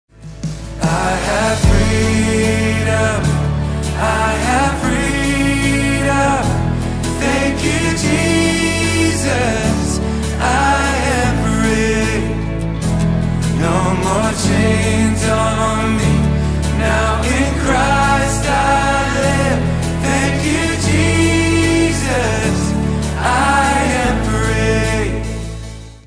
Weekend Worship - July 7th
Weekend Worship for our July 7th holiday service is now set. Click on the song links below to sing along with the songs we will be singing this Sunday! God Bless!